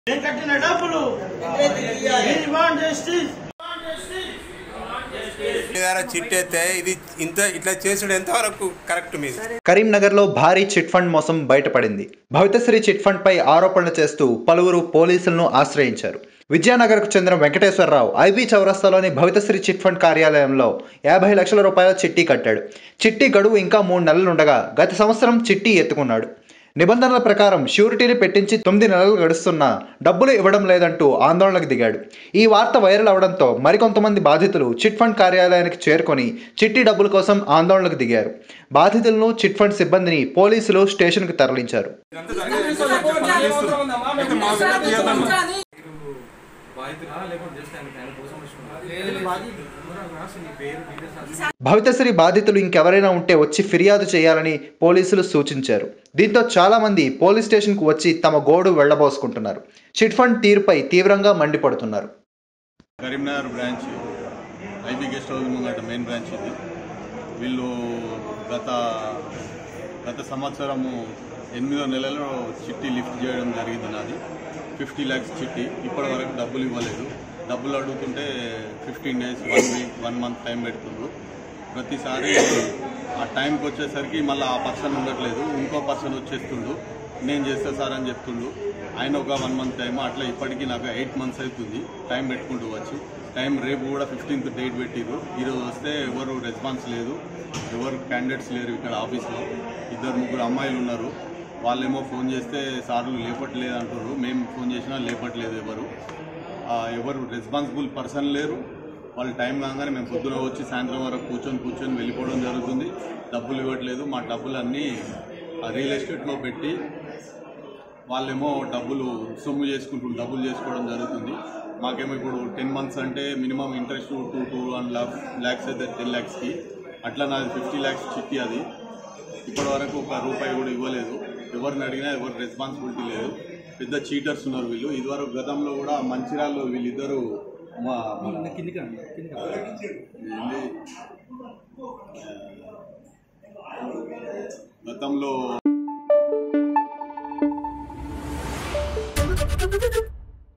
Chit Fund Scam Exposed in Karimnagar: పోలీసులను ఆశ్రయించిన చిట్ ఫండ్ బాధితులు | ABP Desam
Karimnagar లో Bhavita Sri Chitfund పై ఆరోపణలు చేస్తూ పలువురు పోలీసులను ఆశ్రయించారు. అంతకముందు చిట్ ఫండ్ కార్యాలయంలో ఆందోళనకు దిగారు.